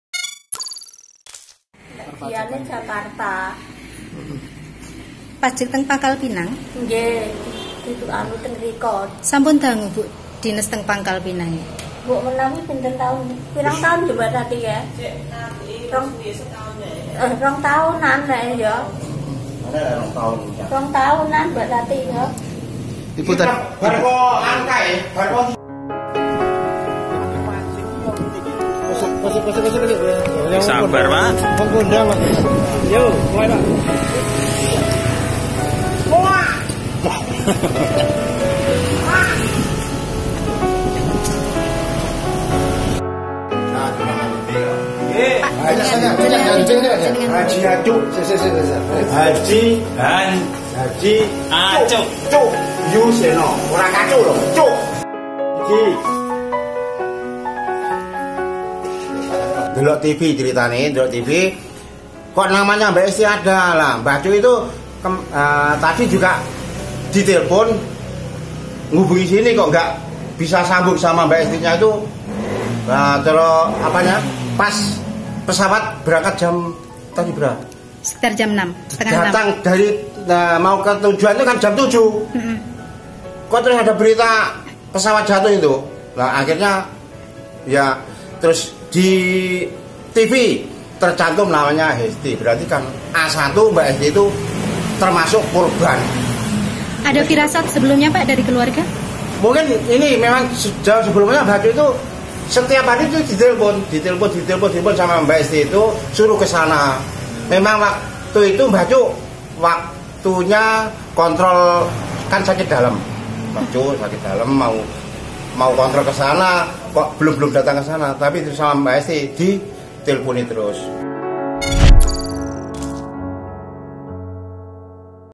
Cerita Tetangga Pegawai Pajak Jadi Korban Lion Air JT-610
Hesti Nuraini tercatat sebagai karyawan di KPP Pratama Bangka sejak dua tahun lalu. Nama Hesti teradapat dalam manifest penumpang pesawat Lion JT-610 yang jatuh di perairan Karawang pada Senin (29/10) pagi.

Terdapat 179 penumpang di pesawat JT 610 dengan rute Jakarta-Pangkalpinang. Pesawat diketahui hilang kontak pada pukul 06.33 WIB sebelum jatuh.